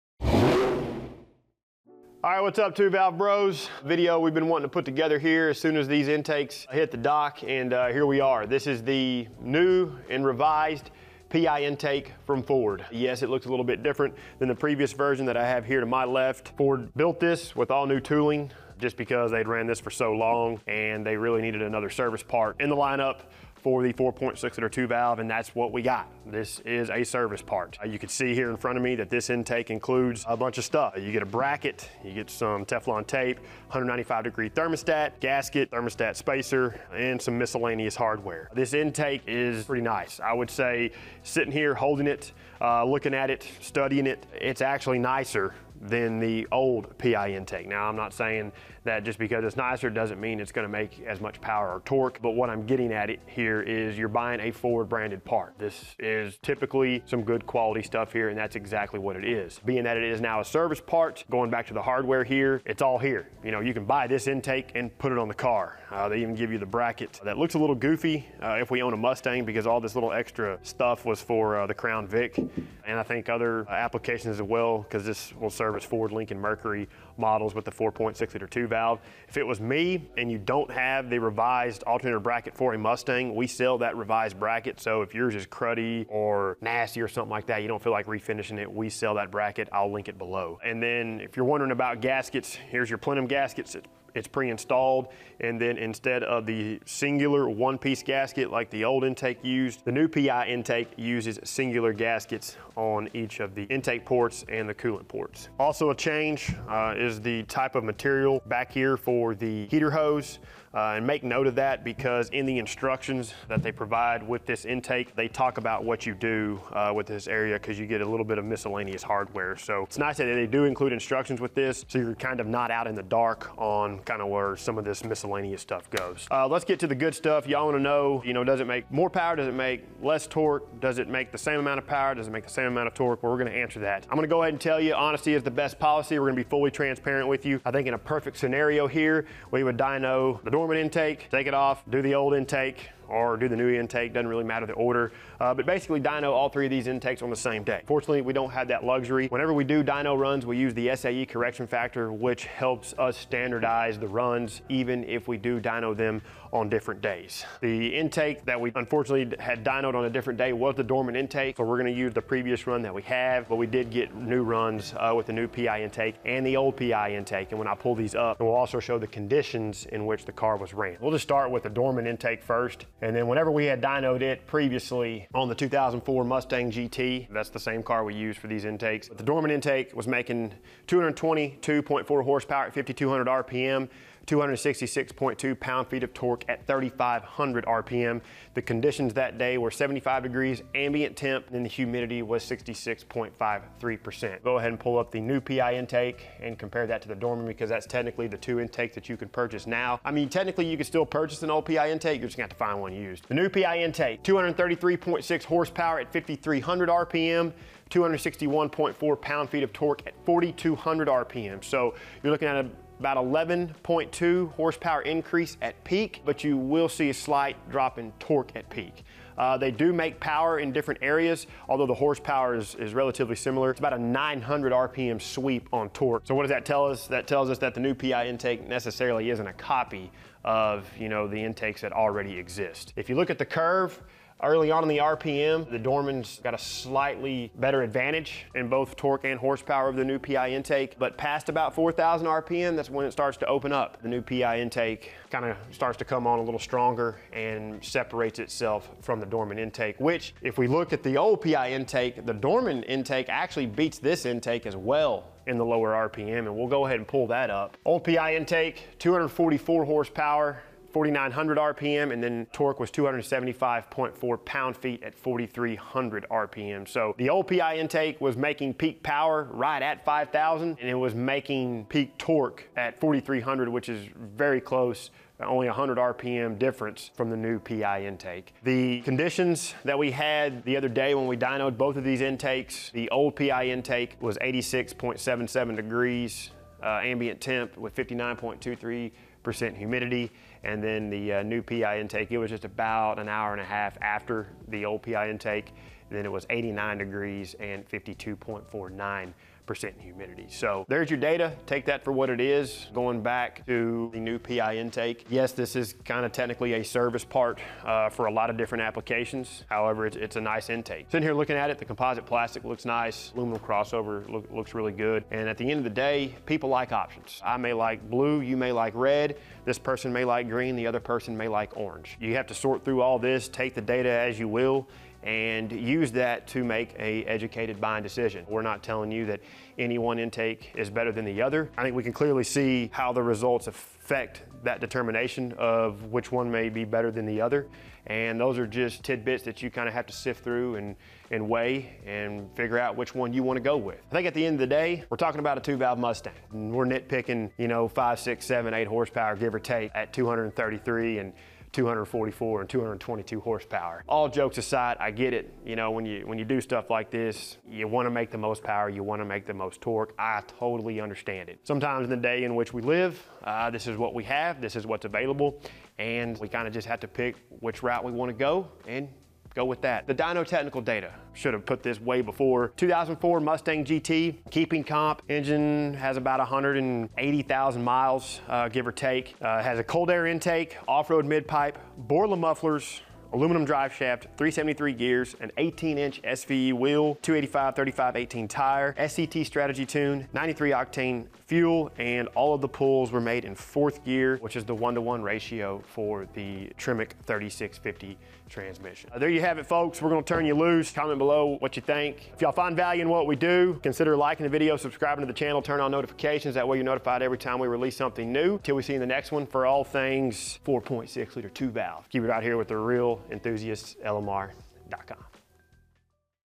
NEW Ford Performance 4.6L 2V PI Intake Manifold (M-9424-P46A) - Dyno Runs & A Closer Look
In this video, Landan takes a closer at the new Ford Performance 4.6L 2V PI intake manifold. We also dive a little deeper with dyno runs of the new intake, the old intake, and the Dorman intake. Enjoy.

Part # M-9424-P46A

*Chapters*
00:00 - New PI intake overview.
02:06 - Dyno results.
05:01 - Final thoughts & outro.